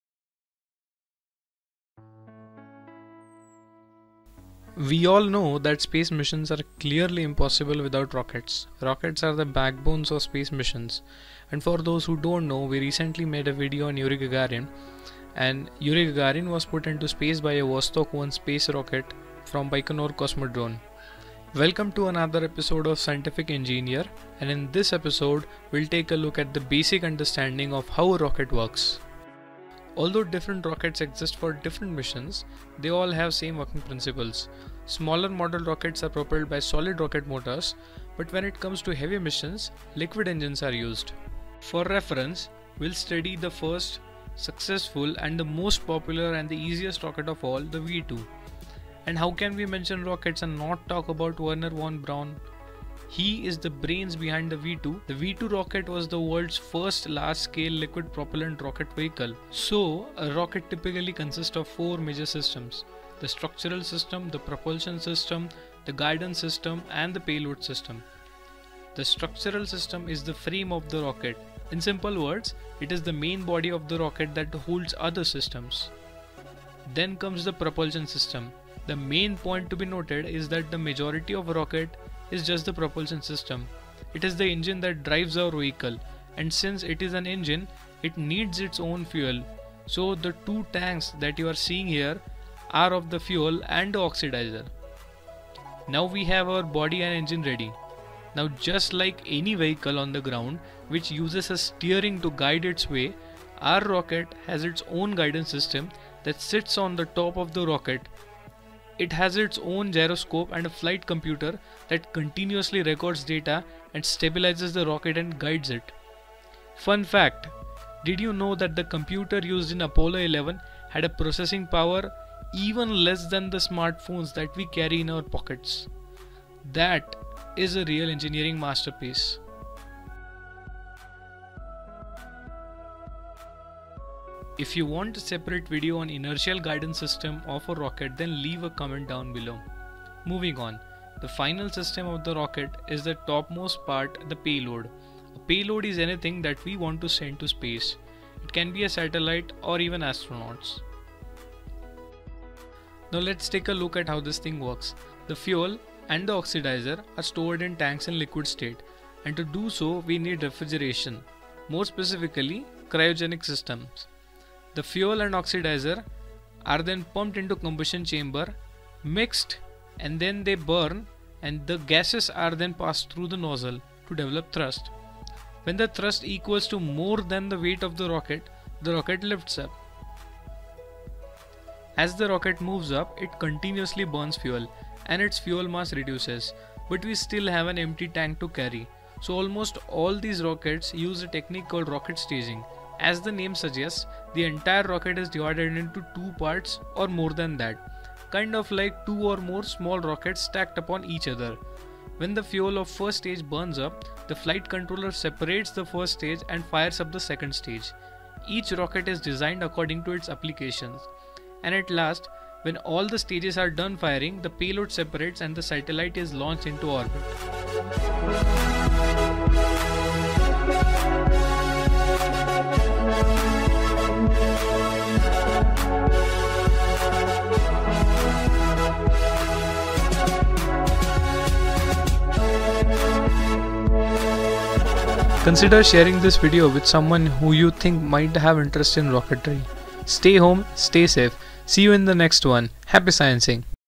Basics of Rocket | Rocket staging | V2 Rocket design | Scientific Engineer
GREETINGS.!!

Hope you all are doing absolutely fine.
So we thought why not make a video on rockets? With all the space race going on,
everyone must have a basic understanding of what a rocket is.
Like most engines, rockets burn fuel. Most rocket engines turn the fuel into hot gas.
The engine pushes the gas out its back. The gas makes the rocket move forward.

A rocket is different from a jet engine. A jet engine needs air to work. A rocket engine
doesn't need air. It carries with it everything it needs. A rocket engine works in space,
where there is no air.Most rockets today operate with either solid or liquid propellants.
There are two main types of rocket engines. Some rockets use liquid fuel. The main
engines on the space shuttle orbiter use liquid fuel. The Russian Soyuz uses liquid fuels.
Other rockets use solid fuels. On the side of the space shuttle are two white solid rocket boosters.
They use solid fuels. Fireworks and model rockets also fly using solid fuels.
SCIENTIFIC ENGINEER

AKASH SHAHADE

Sayali Kuthe

HAPPY SCIENCING .!!

Why are you still reading?